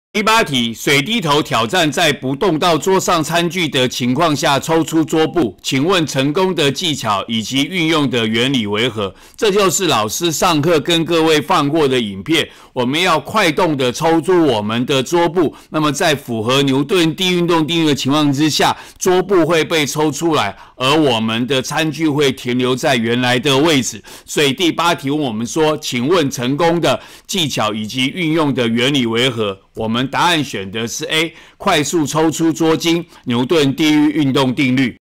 109學年上9年級理化二段第08題
抽出桌布，但桌上餐具不動。問成功技巧與運用原理。